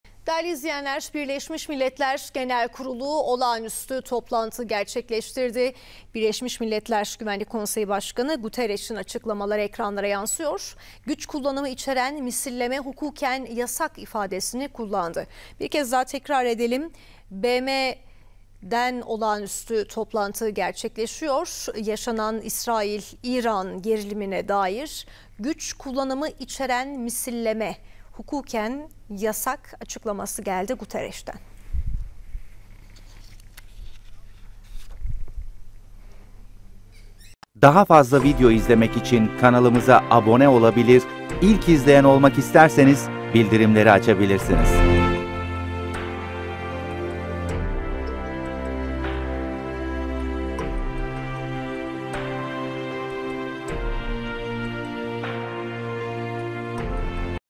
SONDAKİKA BMGK’dan İran-İsrail Toplantısı... "Misilleme Hukuka Göre Yasak!"
BMGK İran-İsrail gerilimi başlıklı toplandı ve misilleme üzerine kritik İran ve İsrail'e mesajlar gönderdi.